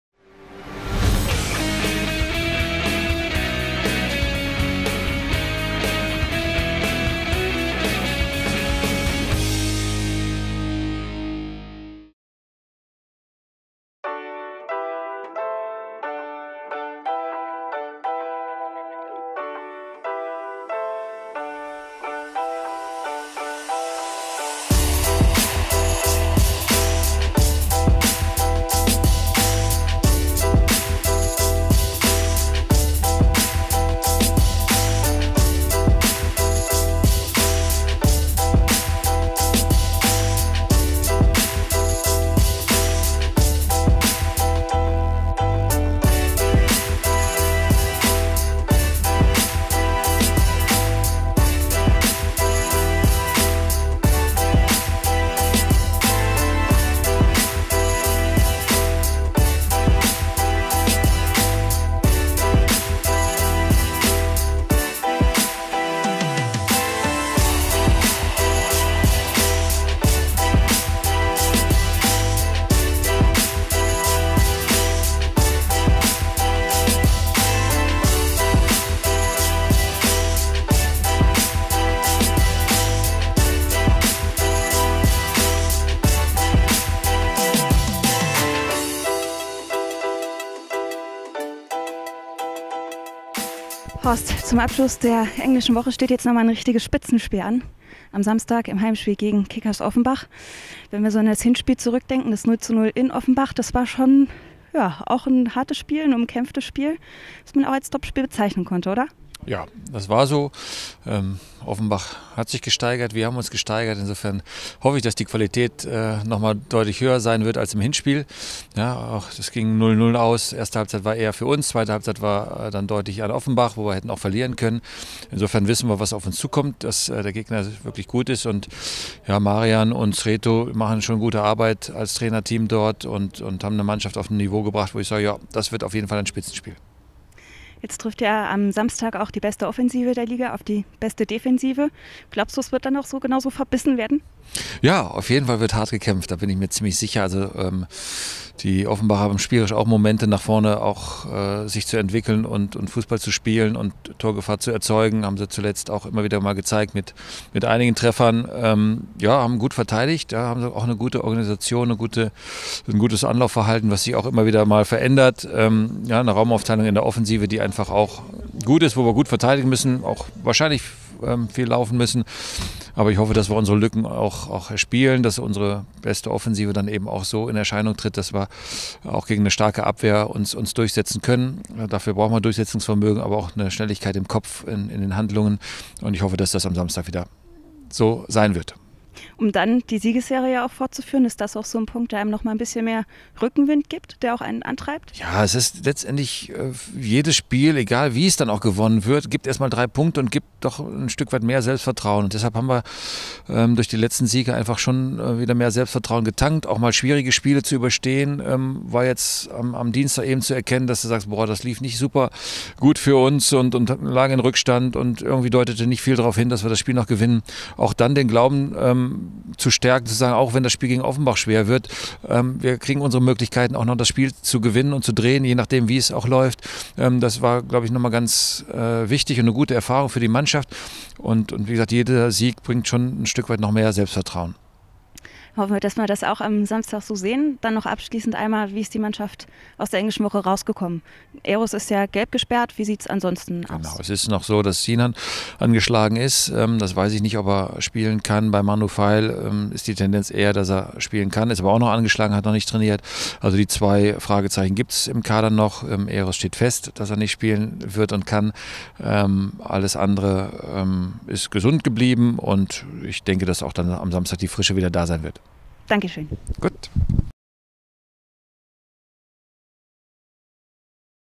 TV Elv // Presse-Talk - SV Elversberg vs. Kickers Offenbach / 08.05.21
Am Samstag empfängt unsere Elv den OFC zum Regionalliga-Spitzenspiel! Den Presse-Talk mit Trainer Horst Steffen gibt's jetzt auf TV Elv.